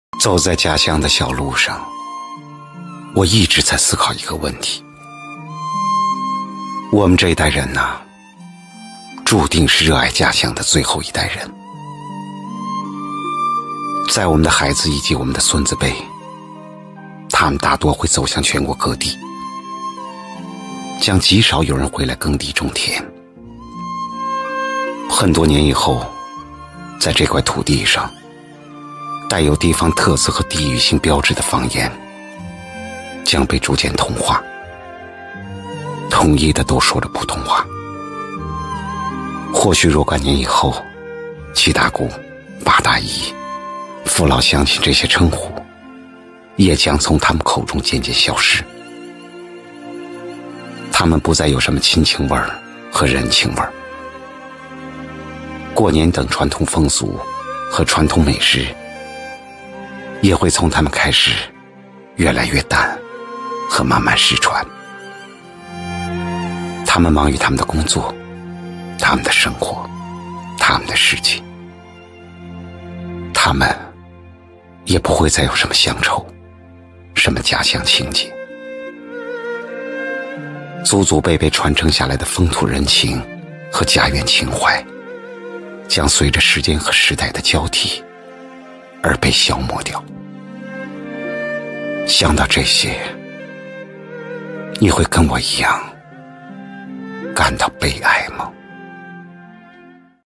走在这样的乡村小道上真的很治愈，仿佛回到了小时候
生活记录,乡村,田园